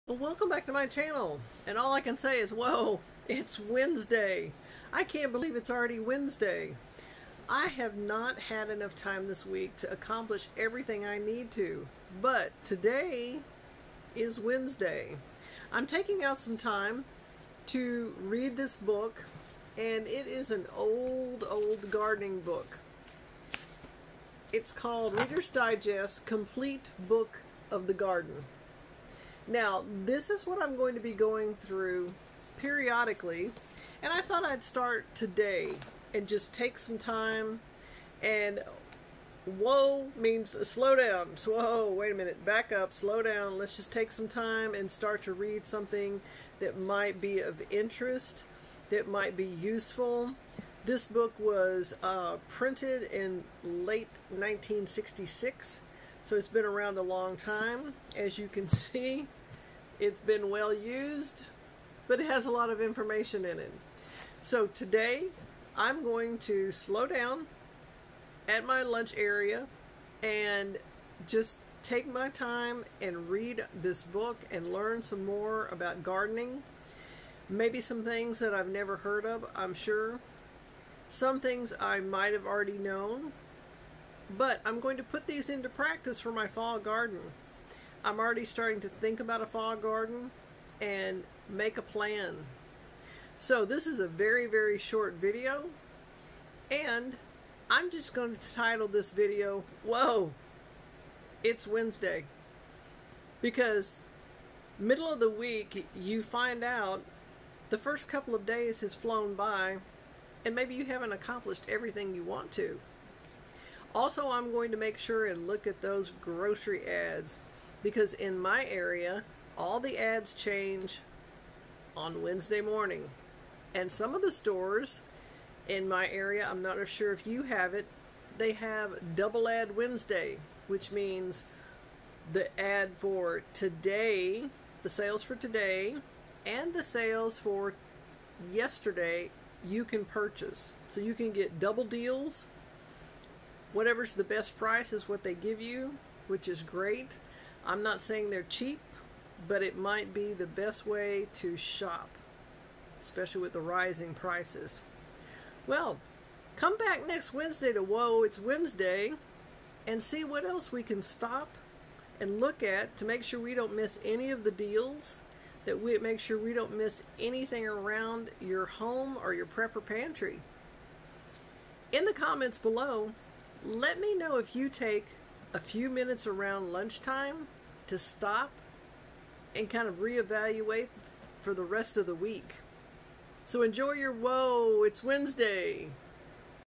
Whoa it's Wednesday! - Take time to read, check your list and prep
Wednesday half way through the week.  Take a little time to check your list, read, check for grocery sales or just make a plan

Enjoy your Wednesday and remember to prep.